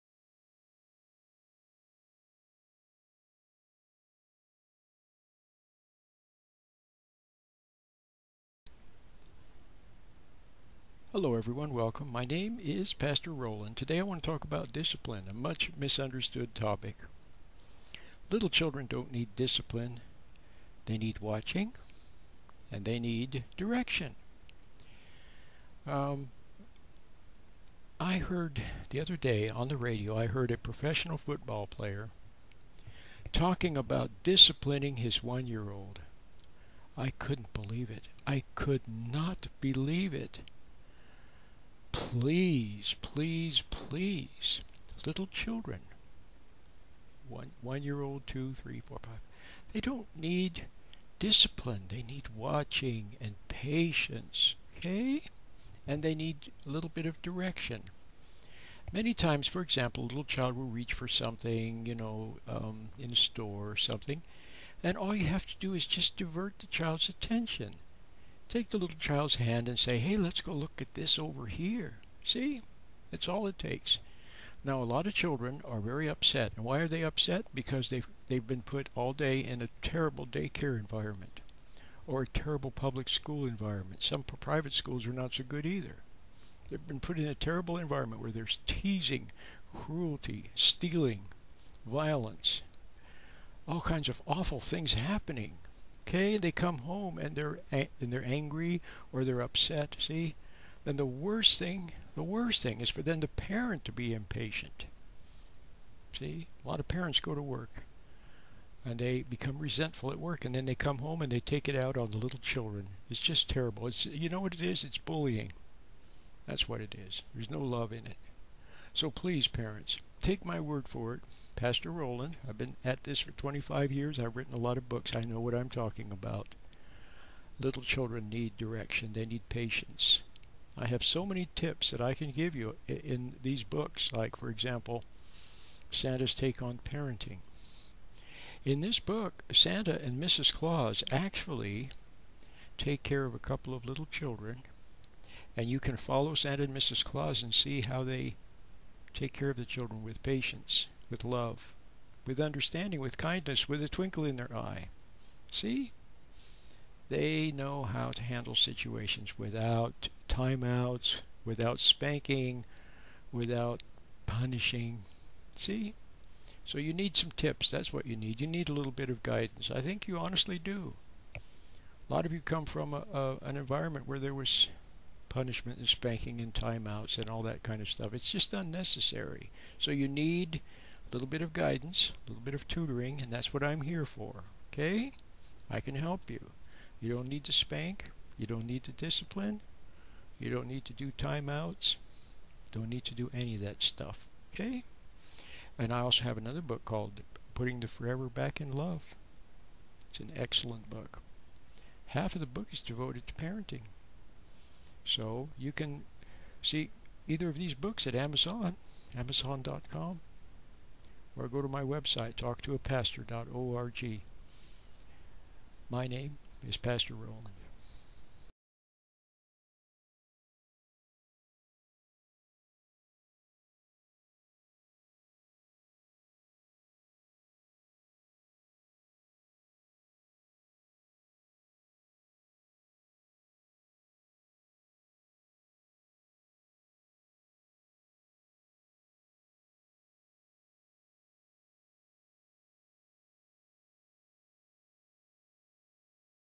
How Do I Discipline My Child?
Parenting with Patience Pastor says little children do not need discipline. They need watching with patience and direction. Pastor Roland has written two books about parenting with patience.